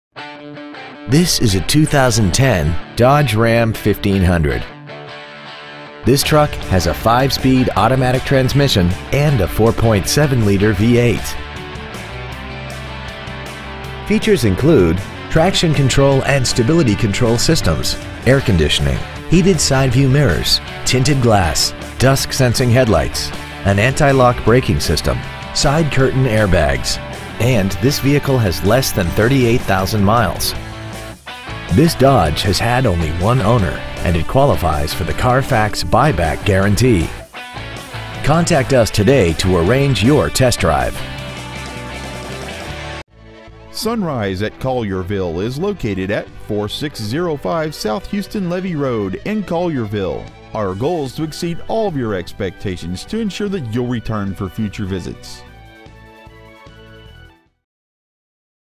2010 Dodge Ram 1500 Collierville TN
Year : 2010
 Make : Dodge
 Model : Ram 1500
 Engine : 4.7L V8
 Trans . : Automatic 5-Speed
 Exterior : Black
 Miles : 37,494
 Interior : Gray
 Stock : M31520A

 4605 S. Houston Levee Rd.